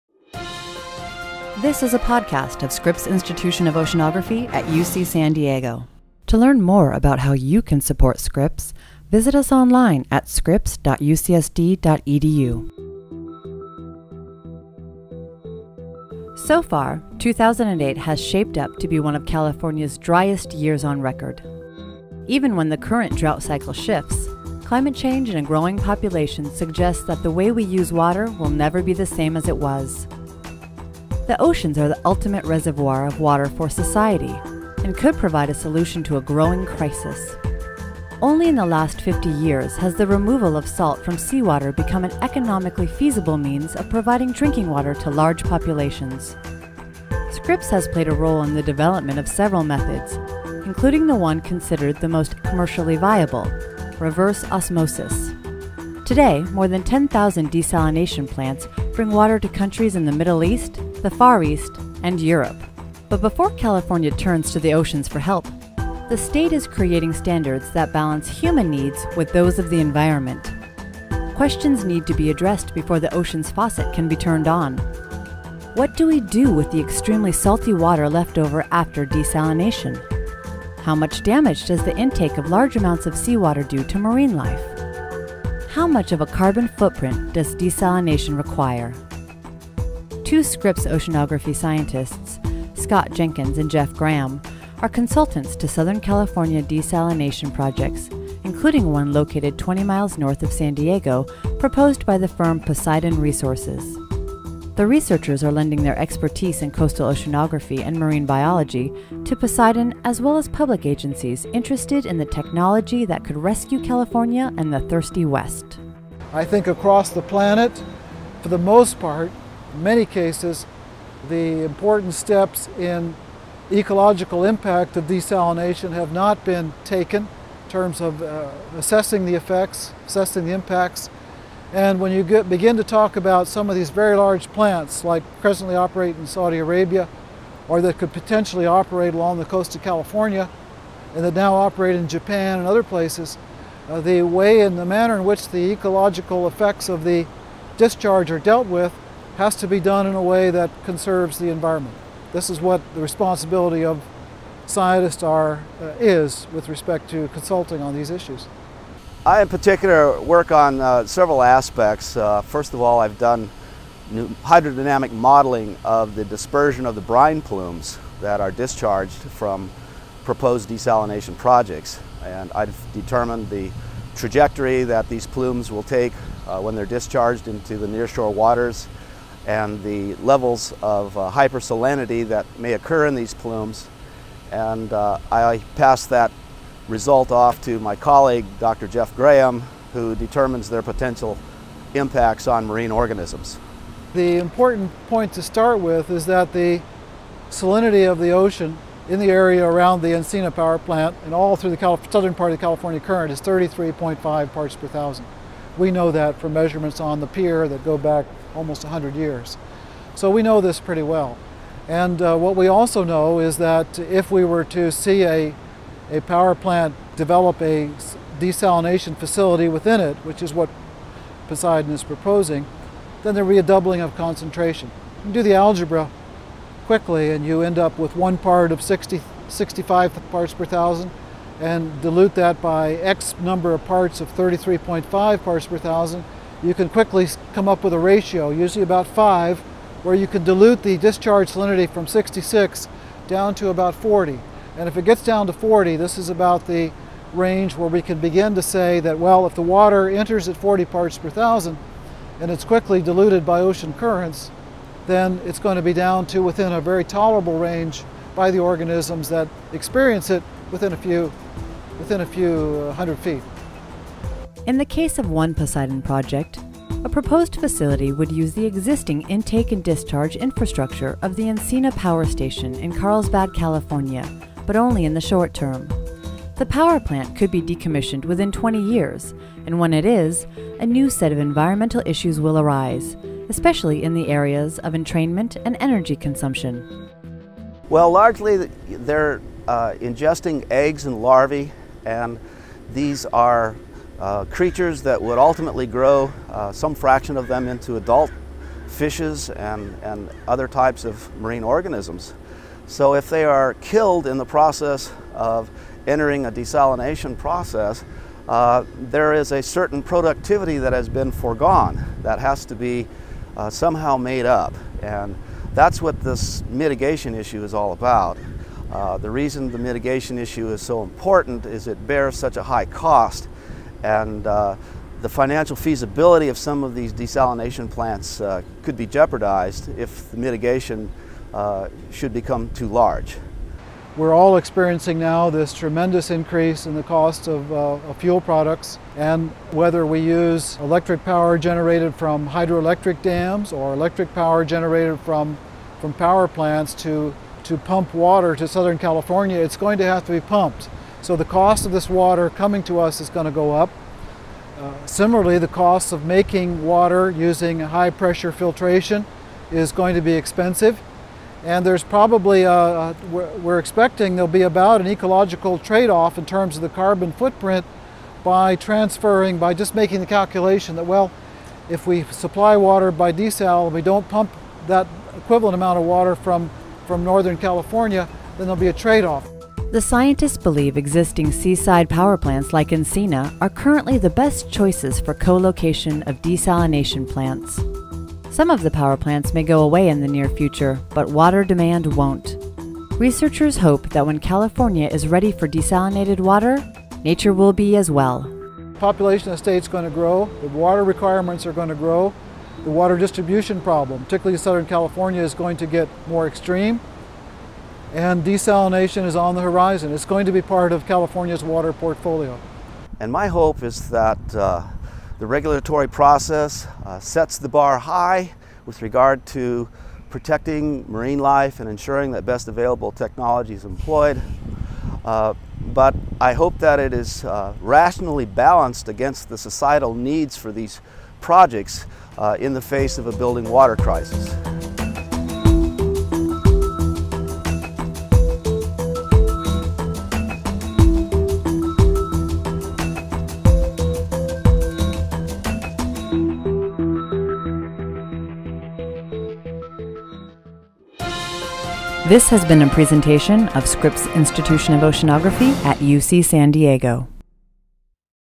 Our First Sip of Seawater
Scripps researchers race to make desalination eco-friendly while there's still time